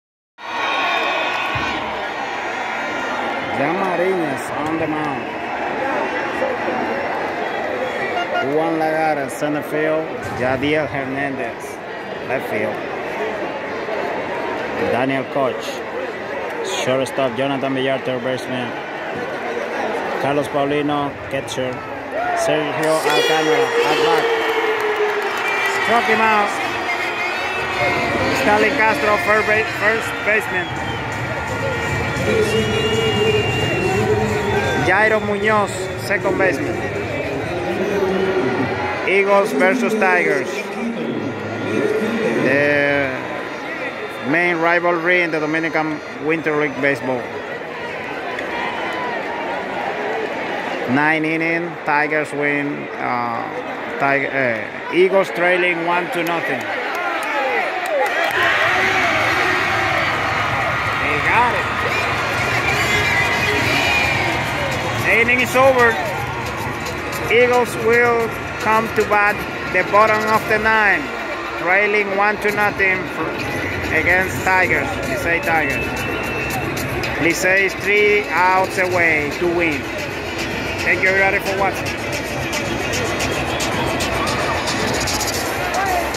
Tigres del Licey Vs Águilas Cibaeñas licey aguilascibaeñas lidom mookie #834
Acción del partido entre los grandes rivales del baseball dominicano.

Steve Moyers lanza en grande y el Licey supera 1-0 a las Aguilas. El zurdo tira 5.0 entradas en blanco, acepta apenas un hit y abanica a 4.

Steve Moyers lució intratable desde el montículo y el relevo azul volvió a imponerse para conducir a los Tigres del Licey a una victoria vía blanqueada 1-0 sobre las Águilas Cibaeñas, en partido celebrado este sábado en el Estadio Cibao.

Moyers, quien estuvo perfecto hasta la cuarta entrada, laboró durante cinco sólidas entradas en blanco de un imparable y cuatro ponches para colocar su récord de por vida contra los de Santiago en 3-1 con 1.86 de efectividad y ahora acumula 29 episodios de seis transferencias y 18 abanicados contra los amarillos.
Fuente: Listín Diario.